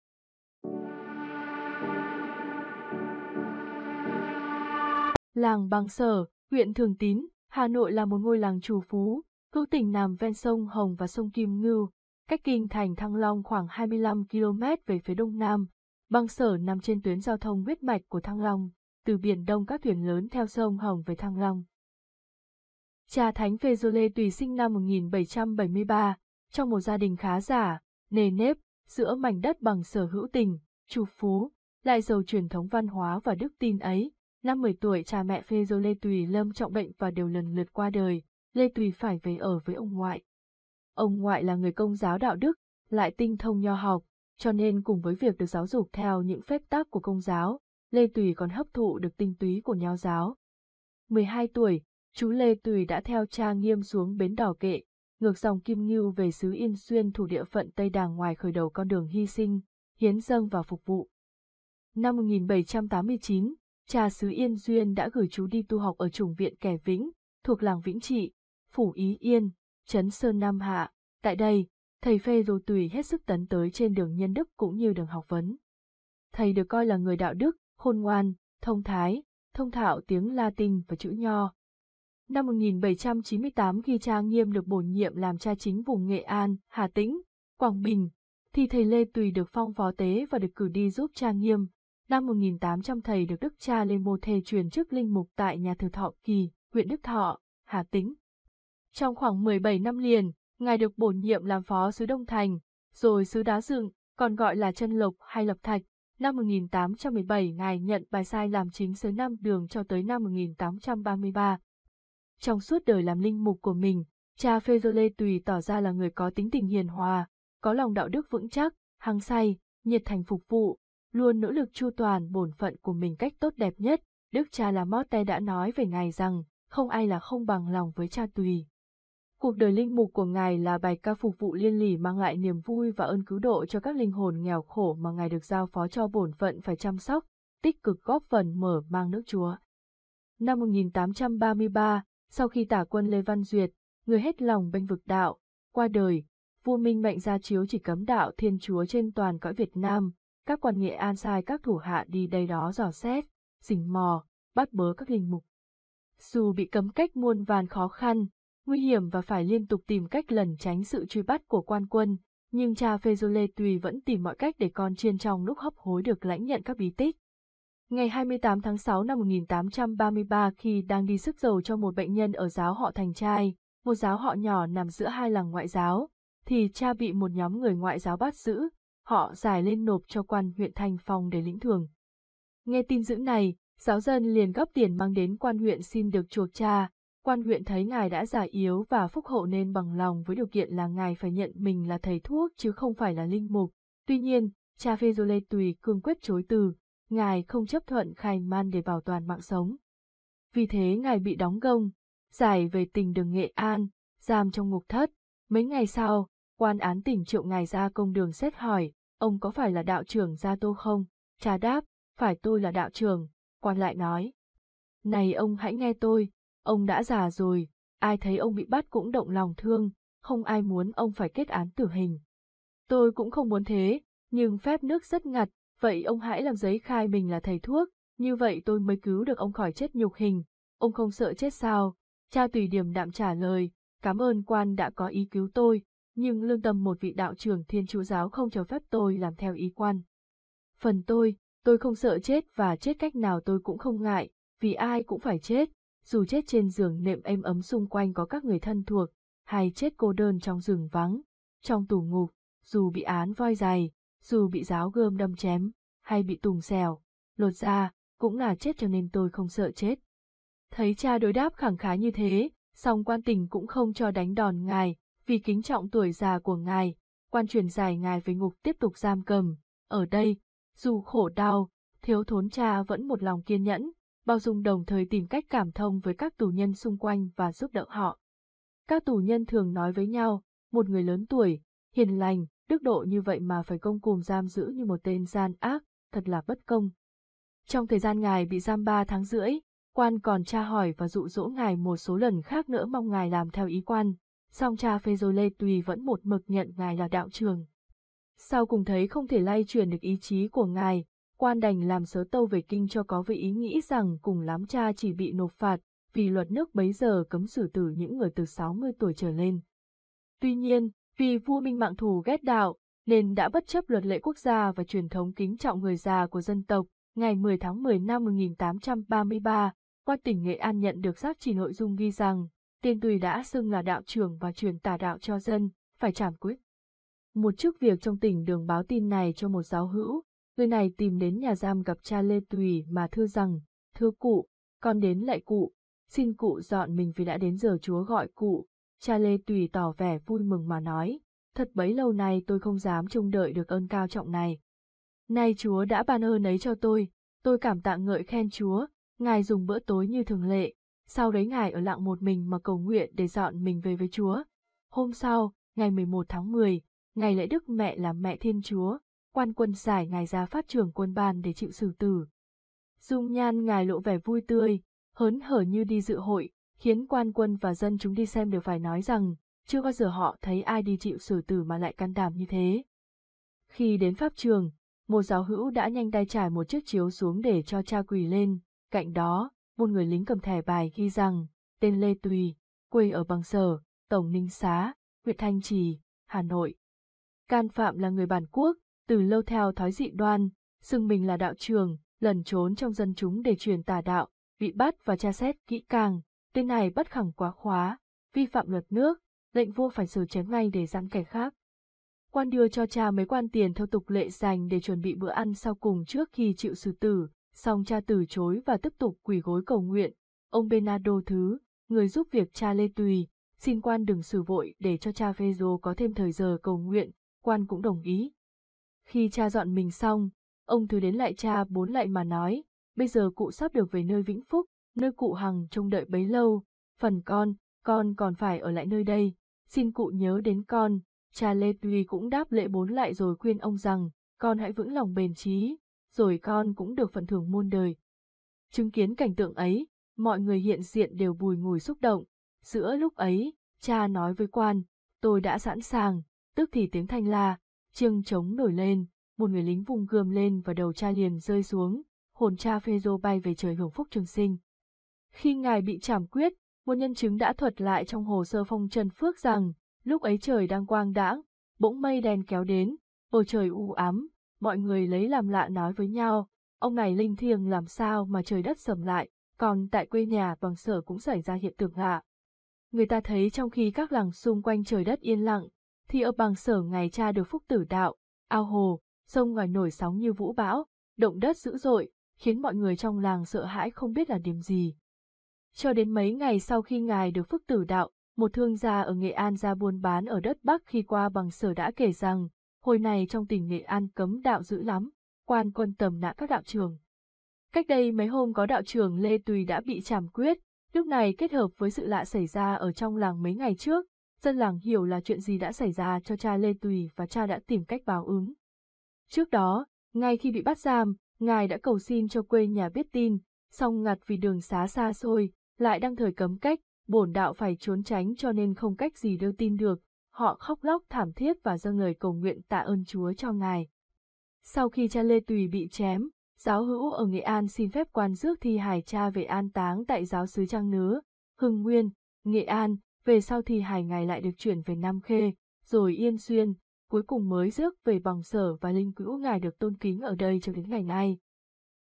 Cuộc đời và con đường lên thánh CHA PHÊ - RÔ LÊ TUỲ
Được chúng con biên soạn và làm thành video để mọi người tìm hiểu về cuộc đời cha  thánh .